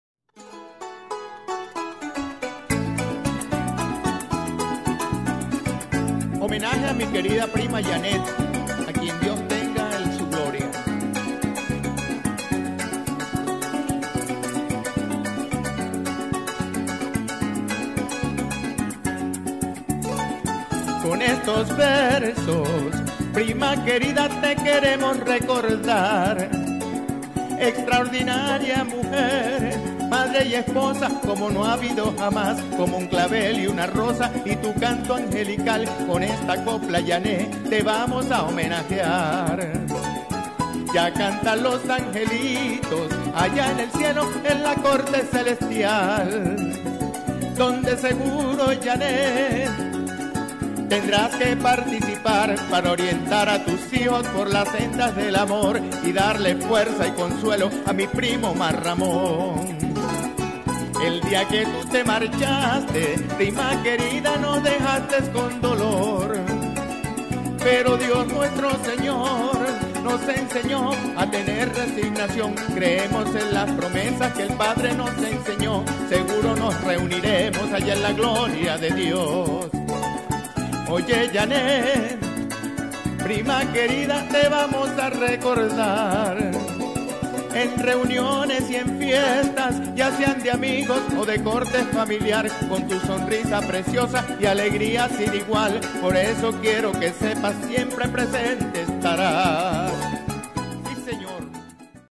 Homenaje a mi querida prima Janet Briceño de Scovino
Kirpa llanera compuesta en homenaje a la partida de mi querida y recordada prima. Ahora en la GLORIA de DIOS Y SIEMPRE PRESENTE EN NUESTROS BELLOS RECUERDOS.
TU PRIMO GONZALO